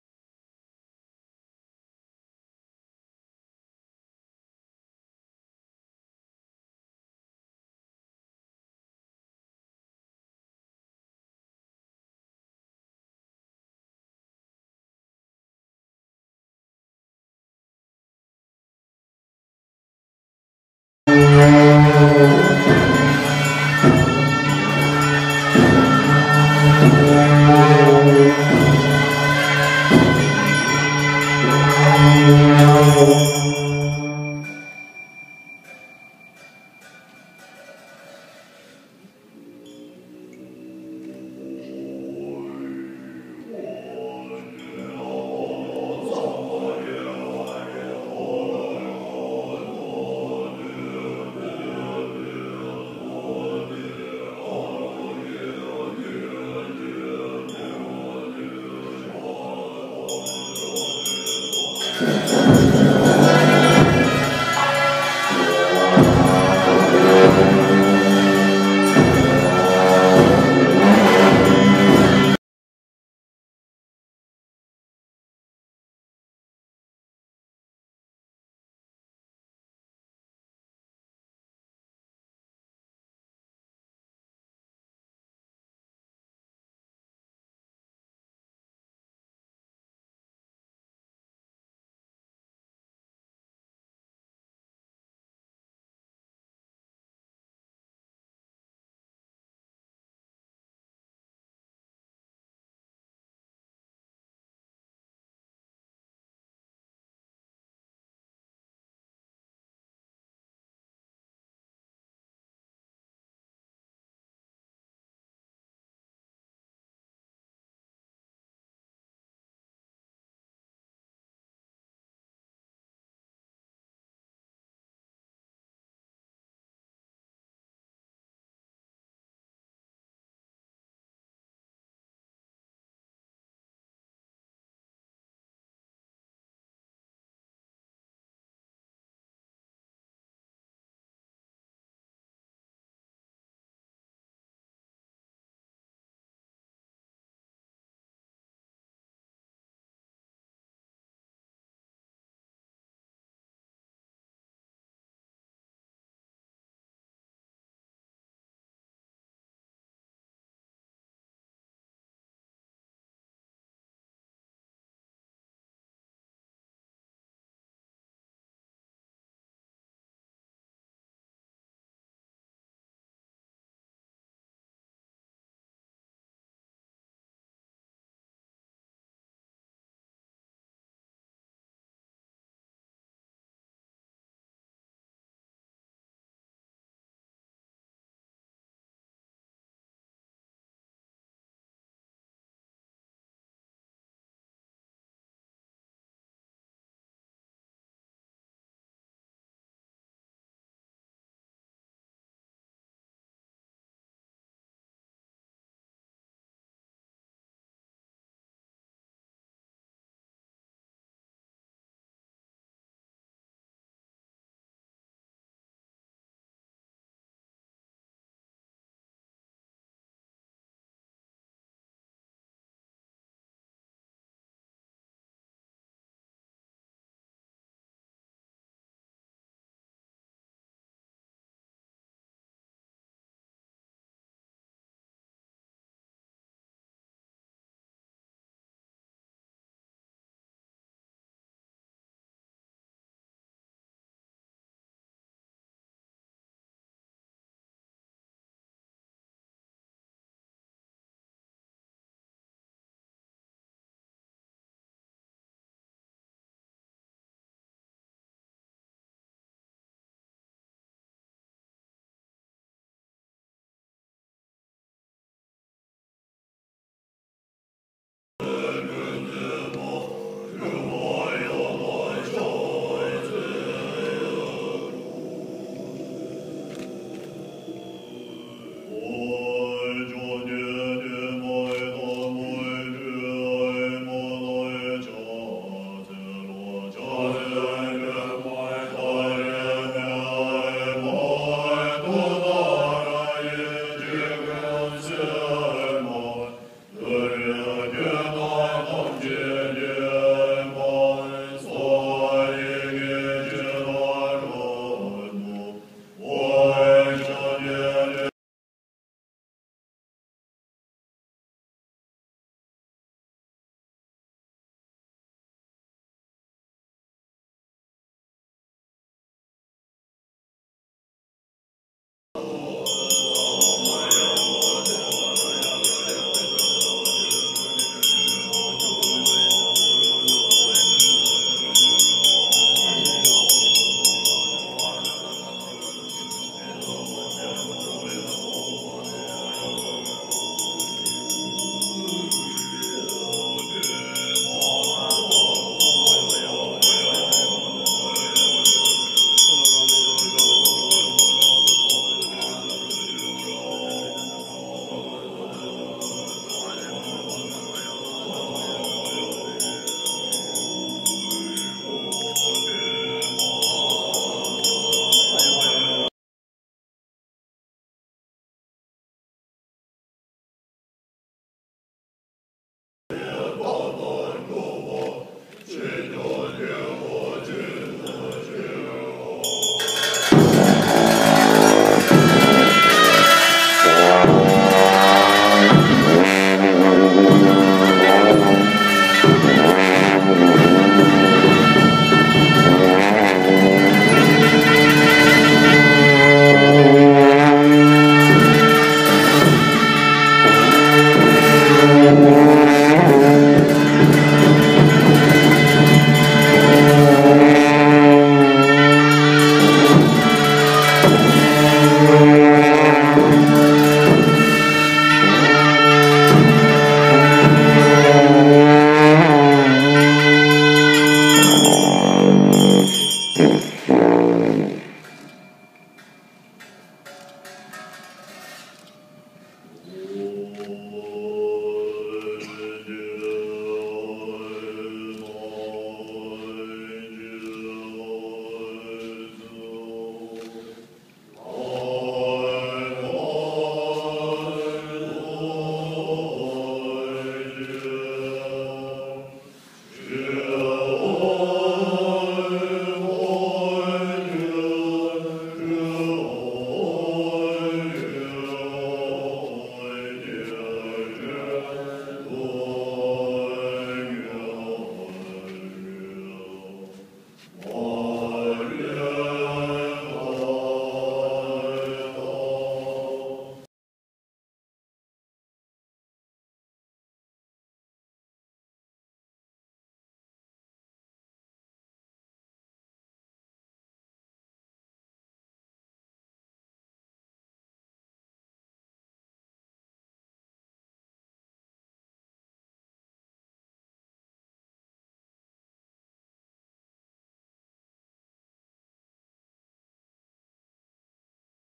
Drepung Loseling Monestary - The Mystical Arts of Tibet
The construction/deconstruction of the mandala and the Crow Collection of Asian Art